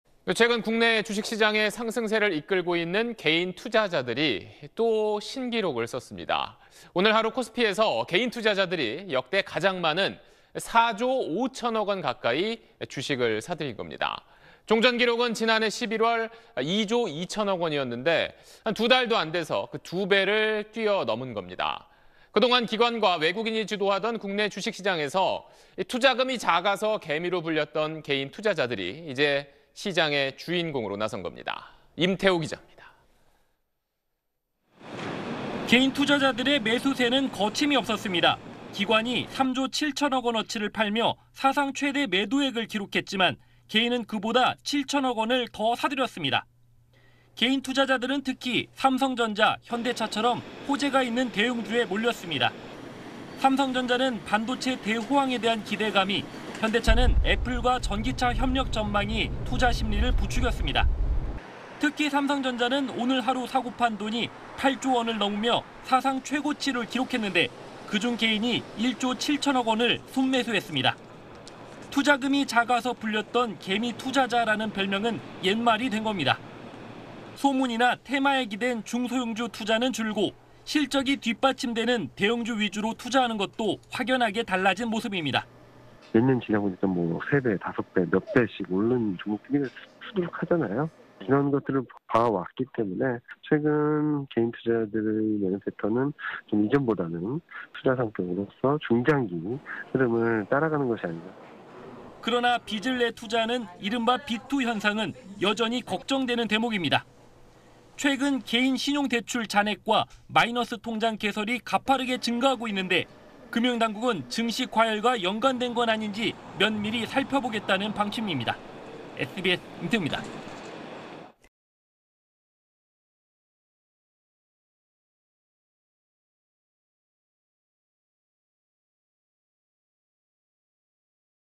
4조 5천억 '묻지마 진격'…달라진 개미 / SBS
최근 국내 주식시장의 상승세를 이끌고 있는 개인투자자들이 또 신기록을 썼습니다. 오늘(11일) 하루 코스피에서 개인 투자자들이 역대 가장 많은 약 4조 5천억 원 가까이 주식을 사들인 겁니다. 종전 기록은 지난해 11월, 2조 2천억 원이었는데 두 달도 안 돼서 그 두 배를 뛰어넘은 겁니다. 그동안 기관과 외국인이 주도하던 국내 주식 시장에서 투자금이 작아서 개미로 불렸던 개인투자자들이 이제 시장의 주인공으로 나선 겁니다.
임태우 기자입니다.